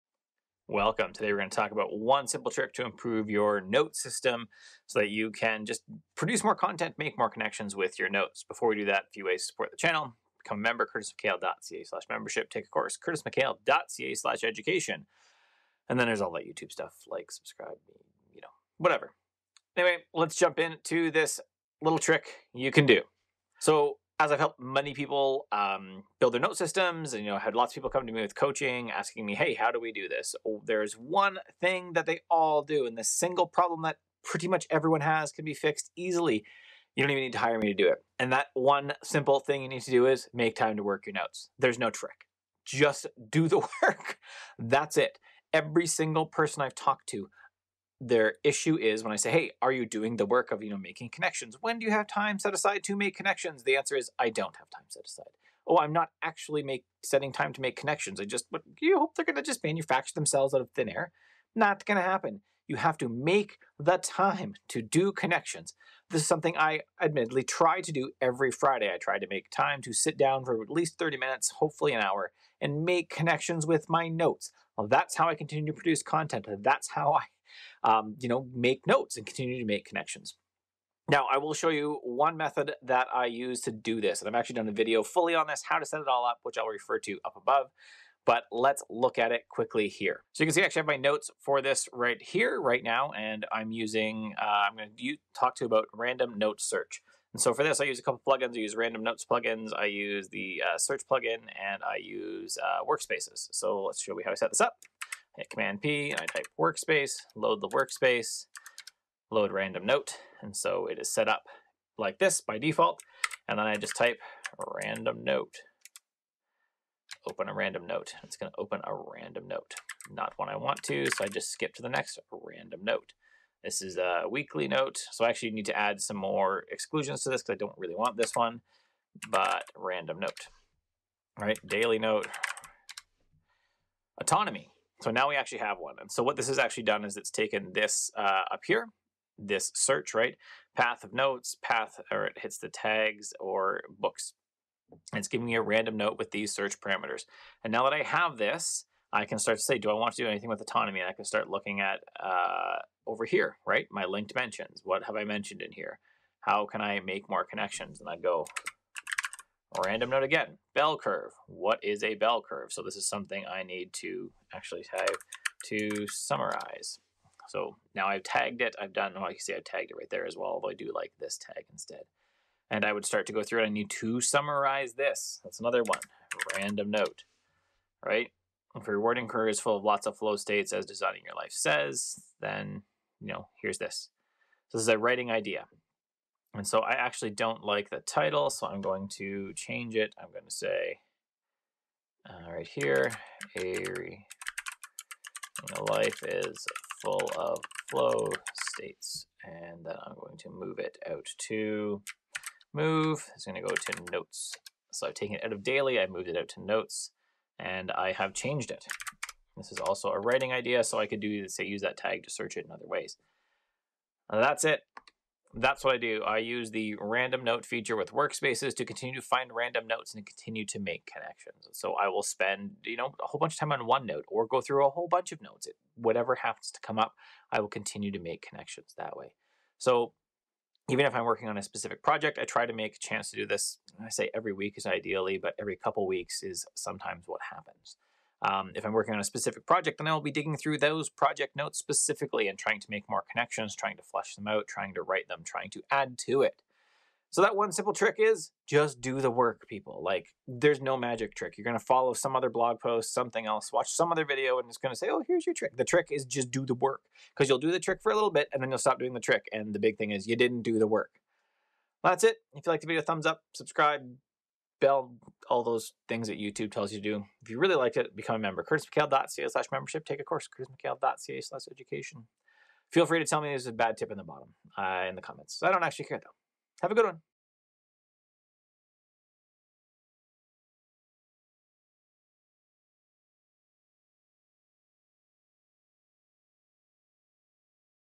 One Trick to Make Connections with Your Notes
Here is my one simple trick to make connections on notes.

## Courses

## My Gear